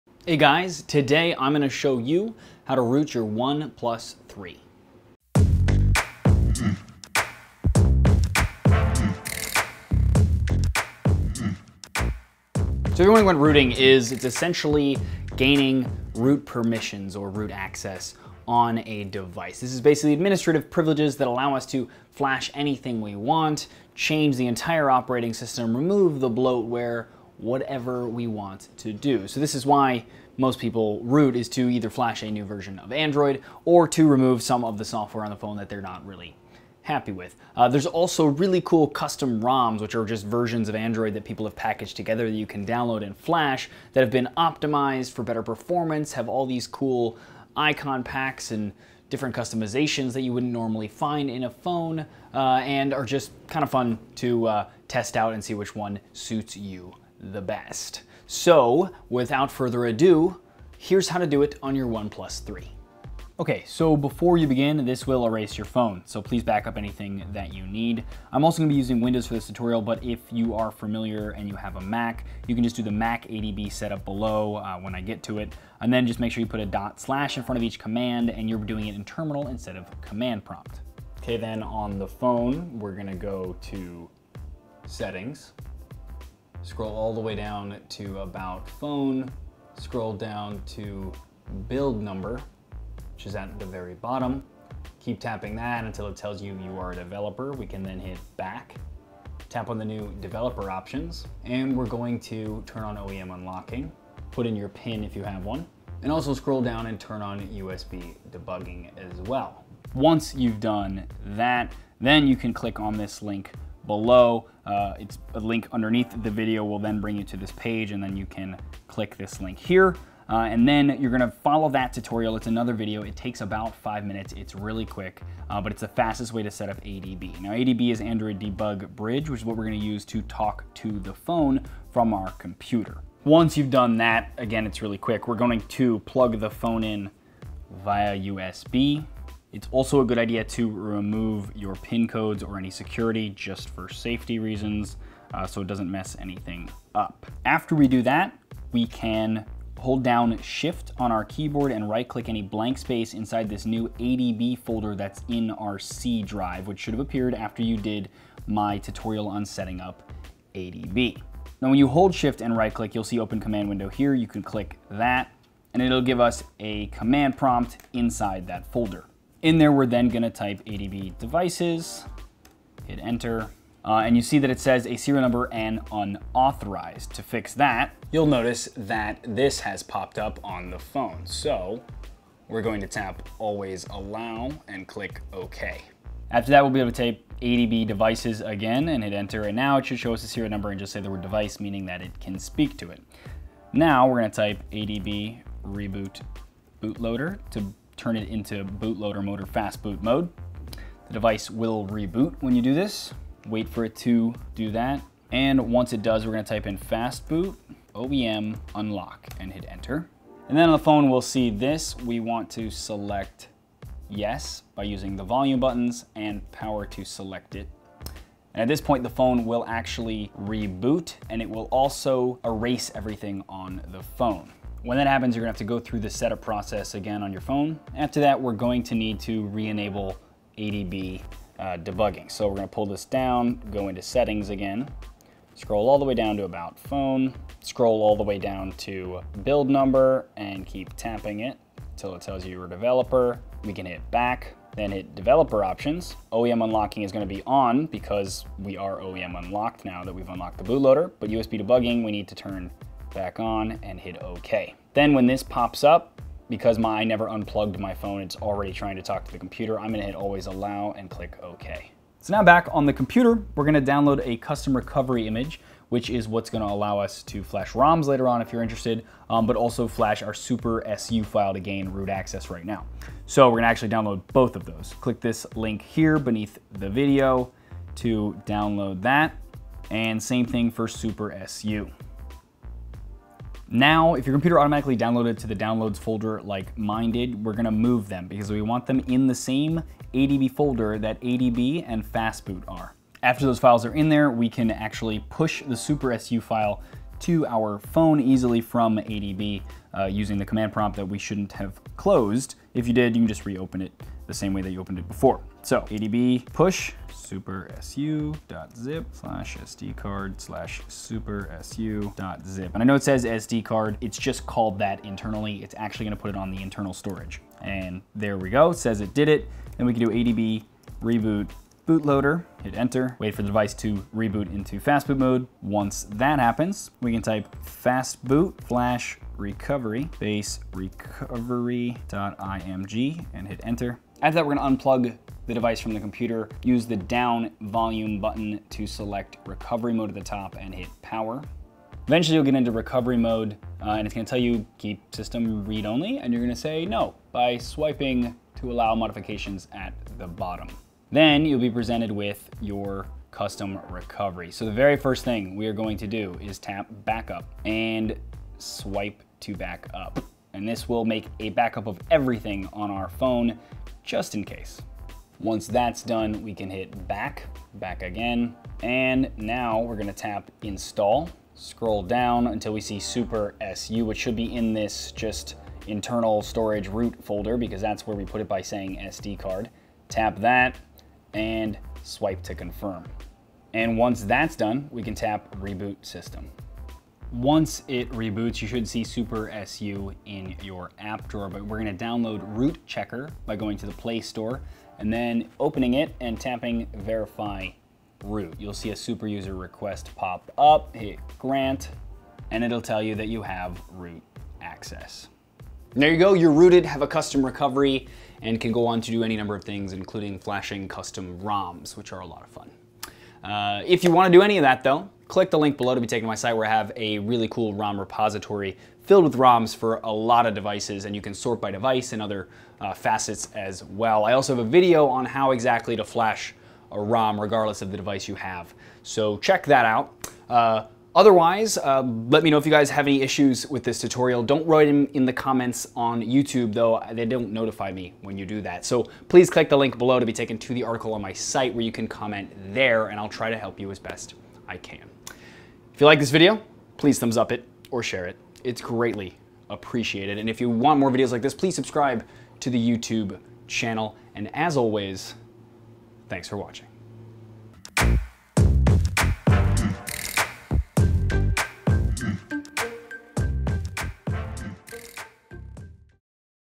How to Root the OnePlus 3
Video Description:
The OnePlus 3 is an amazing phone. Slick metal design, 6GB of RAM (not a typo and more than a PC in my house has), and a clean version of Android that just flies. Best of all? All of this is only $399 factory unlocked from OnePlus' site.

Now, even though it's a great phone, there is always room for improvement. One such way to improve an Android phone is to root it. If you root the OnePlus 3, you'll get access to root-only apps in the Play Store, be able to flash a custom recovery and even flash custom ROMs (think new versions of Android with custom skins, performance tweaks, etc.).

Thanks to OnePlus' own lax attitude towards rooting (this is a company that used to use Cyanogen, master of rooting, on their devices), it's pretty easy to do and easy to undo as well. Here's how to root the OnePlus 3.

Head here for the latest jailbreaking tutorials and jailbreak tweaks!

Set your phone free! You can buy unlock codes to allow you to use any carrier with your phone, head here!